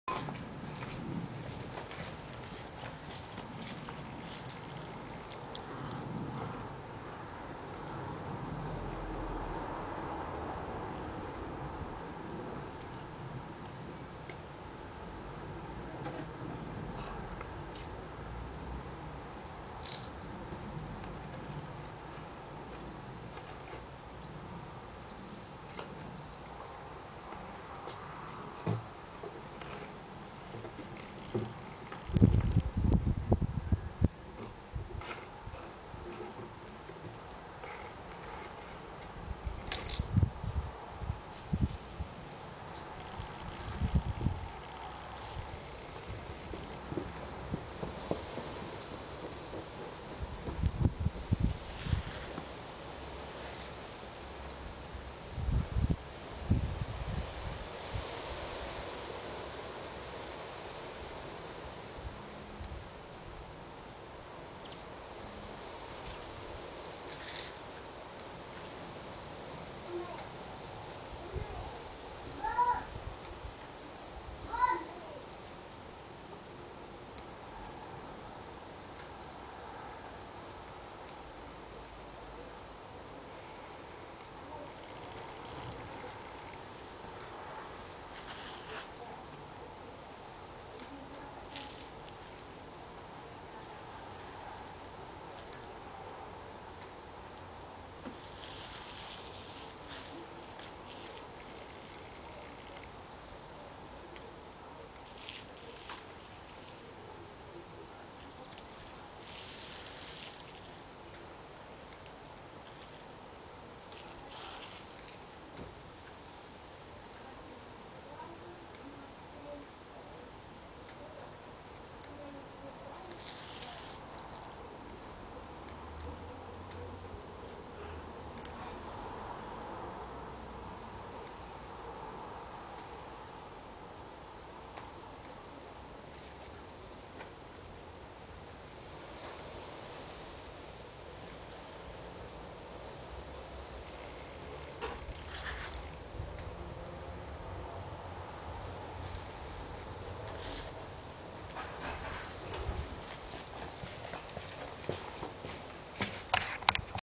Chen foundation form: Back view - From Dragon River Tai Chi school - Nottingham
This is the foundation form from the back - easier to follow but you can't see what my hands are doing so easily.